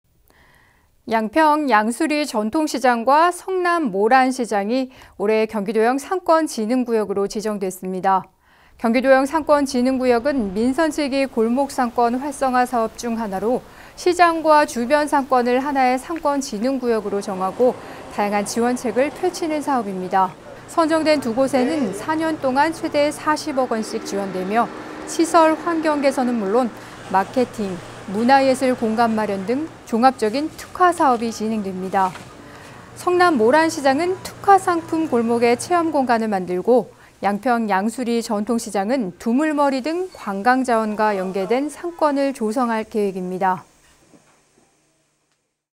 양평 양수리∙성남 모란시장, 상권진흥구역 지정
양평 양수리 전통시장과 성남 모란시장이 올해 ‘경기도형 상권진흥구역’으로 지정됐습니다.

‘경기도형 상권진흥구역’은 민선 7기 골목상권 활성화 사업 중 하나로, 시장과 주변 상권을 하나의 상권진흥구역으로 정하고 다양한 지원책을 펼치는 사업입니다.

선정된 2곳에는 4년 동안 최대 40억 원씩 지원되며, 시설∙환경 개선은 물론 마케팅, 문화예술 공간 마련 등 종합적인 특화사업이 진행됩니다.

성남 모란시장은 특화상품 골목에 체험공간을 만들고, 양평 양수리 전통시장은 두물머리 등 관광자원과 연계된 상권을 조성할 계획입니다.

영상취재 : 류민호 이효진 , 영상편집 : 강윤식